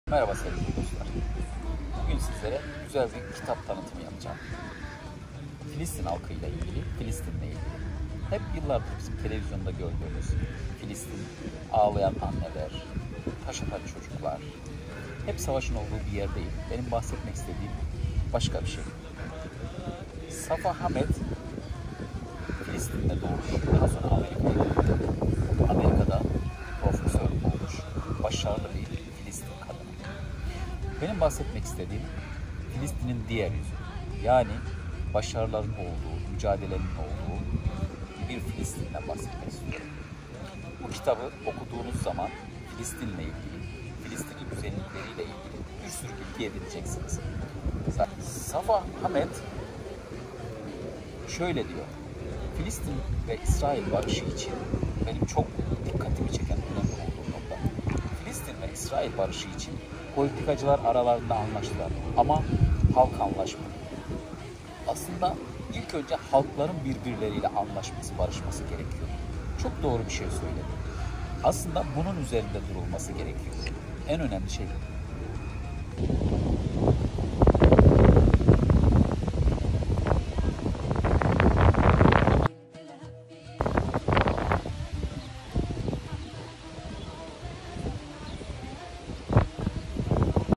Filistin ve israil halklarının barışı adına.”Safa hamed LOVE LIFE & PRESS ON WITH PEACE
Filistin ve israil halklarının barışı adına yol gösterici bir kitap.
“İsrail ve filistinli politikacılar el sıkışıp anlaştılar ama halkların birbirleriyle anlaşıp el sıkışmalarına olanak sağlamadılar bu yüzden barış gelmedi.”
Safa hamed LOVE LIFE & PRESS ON WITH PEACE